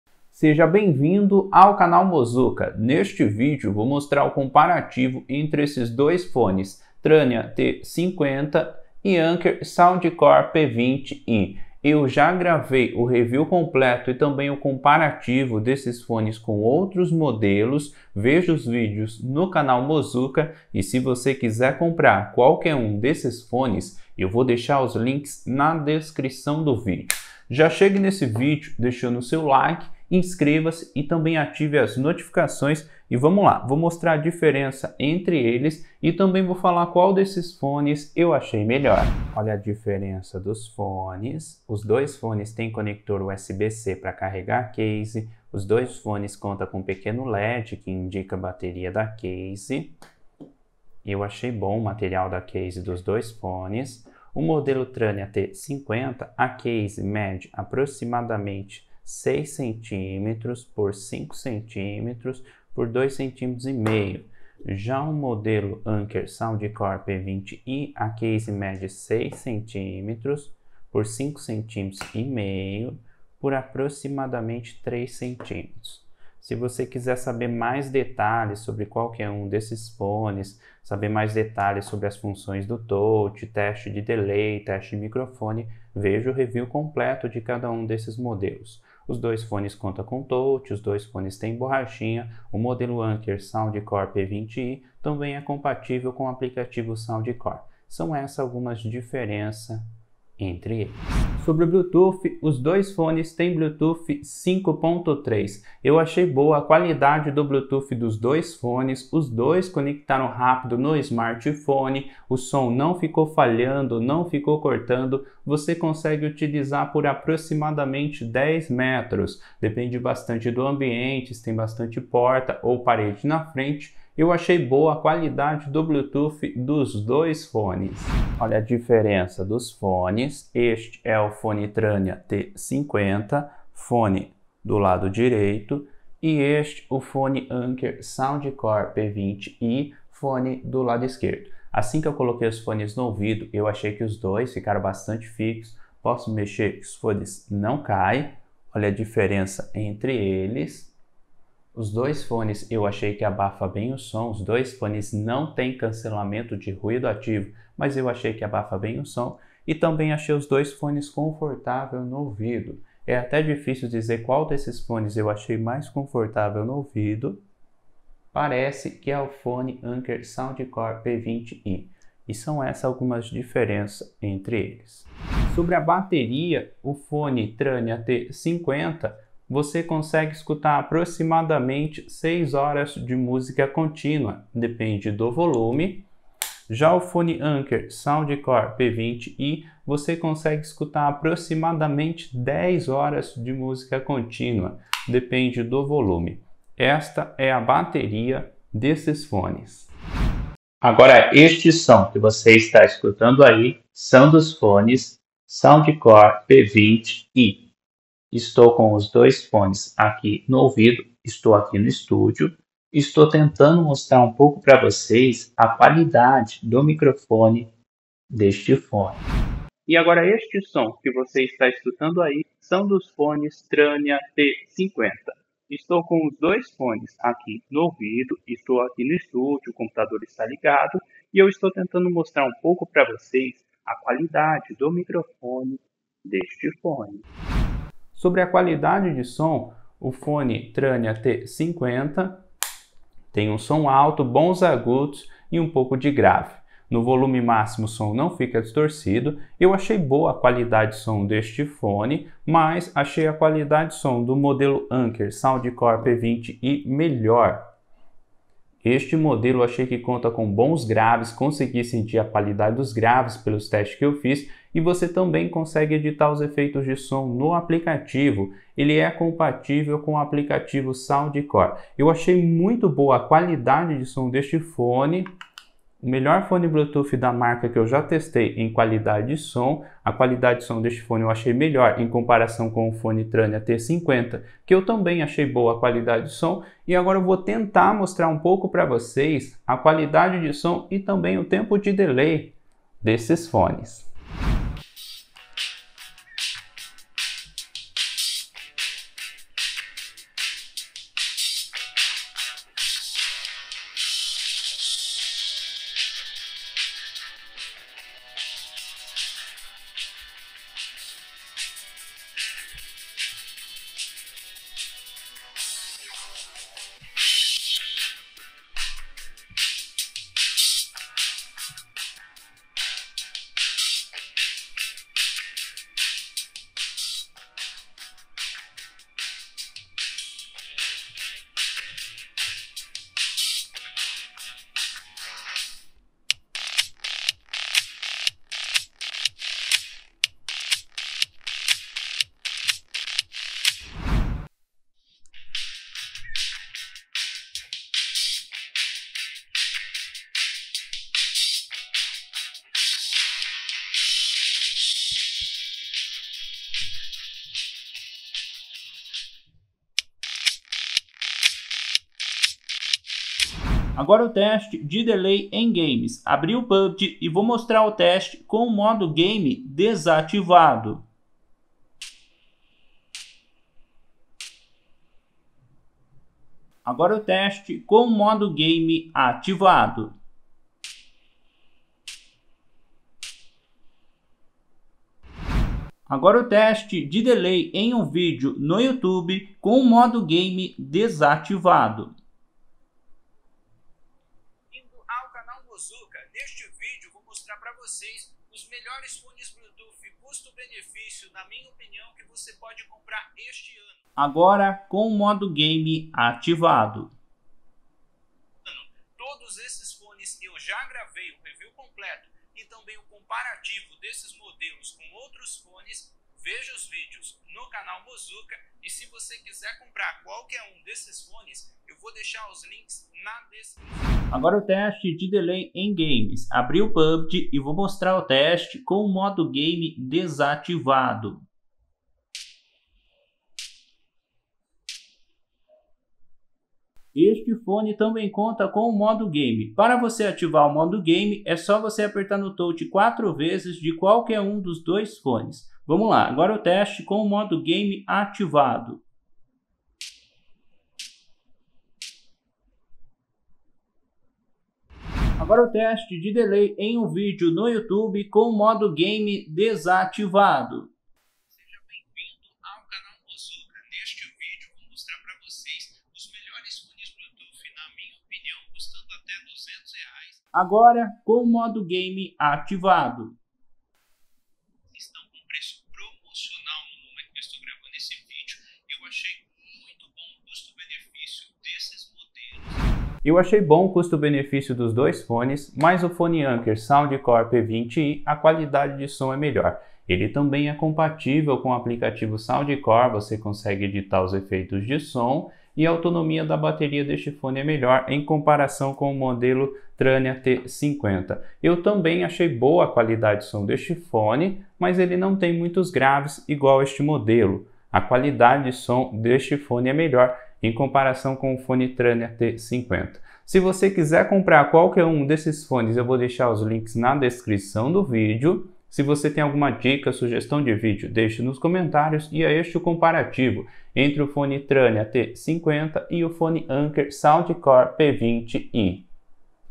Anker P20i vs Tranya T50 Comparativo!! Qual fone tem melhor SOM, DELAY E BATERIA?
🛒 Tranya T50 (Amazon)

(00:00) Introdução
(00:39) Diferença entre os fones
(01:41) Bluetooth
(02:08) Como fica no ouvido
(02:55) Bateria
(03:20) Teste de Microfone Anker P20i
(03:39) Teste de Microfone Tranya T50
(03:57) Qualidade de Som
(04:54) Teste de Som Anker P20i
(05:44) Teste de Som Tranya T50
(06:03) Teste de Delay Anker P20i
(07:04) Teste de Delay Tranya T50
(09:03) Qual fone é melhor?